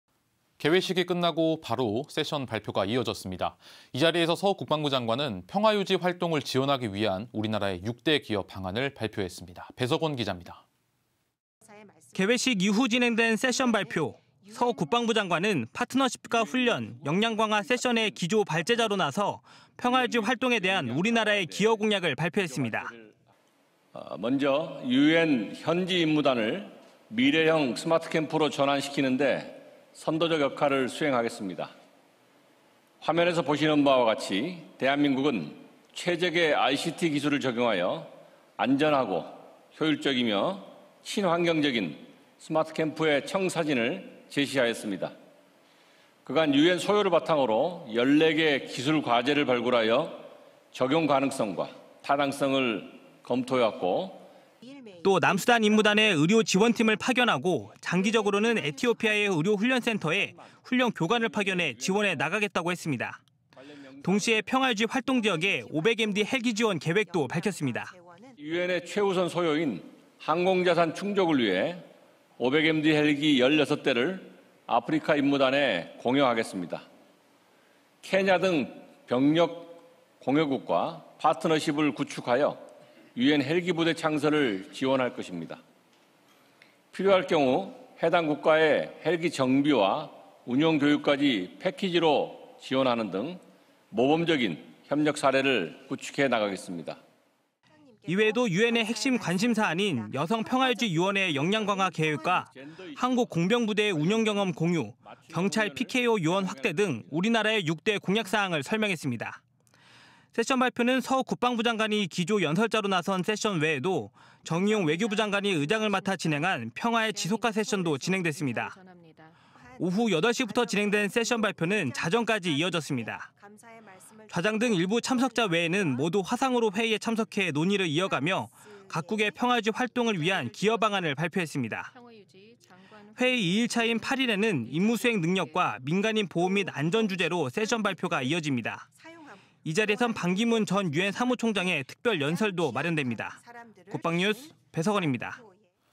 [2021 서울 유엔 평화유지 장관회의] 서욱 국방부장관, 우리나라 6대 PKO 기여 공약 발표
개회식이 끝나고 바로 세션 발표가 이어졌습니다.
이 자리에서 서욱 국방부장관은 평화유지활동을 지원하기 위한
우리나라의 6대 기여 방안을 발표했습니다.
배석원 기잡니다.

개회식 이후 진행된 세션 발표.

서욱 국방부장관은
 파트너십과 훈련, 역량강화 세션에 기조발제자로 나서 평화유지 활동에 대한 우리나라의 기여 공약을 발표했습니다.

또 남수단임무단에 의료지원팀을 파견하고
 장기적으로는 에티오피아의 의료훈련센터에
 훈련교관을 파견해 지원해 나가겠다고 했습니다.

동시에 평화유지 활동 지역에 500MD 헬기 지원 계획도 밝혔습니다.

이외에도 UN의 핵심 관심 사안인 여성 평화유지요원의 역량 강화 계획과
 한국 공병부대의 운용 경험 공유, 경찰 PKO요원 확대 등
 우리나라의 6대 공약사항을 설명했습니다.

세션 발표는 서욱 국방부장관이 기조연설자로 나선 세션 외에도
 정의용 외교부장관이 의장을 맡아 진행한
‘평화의 지속화 세션’도 진행됐습니다.

오후 8시부터 시작된 세션 발표는 자정까지 이어졌습니다.

좌장 등 일부 참석자 외에는 모두 화상으로 회의에 참석해 논의를 이어가며
 각 국의 평화유지 활동을 위한 기여 방안을 발표했습니다.

회의 2일 차인 8일에는 ‘임무수행능력’과 ‘민간인 보호 및 안전’ 주제로 세션 발표가 이어집니다.

이 자리에선 반기문 전 유엔사무총장의 특별연설도 마련됩니다.

국방뉴스 배석원입니다.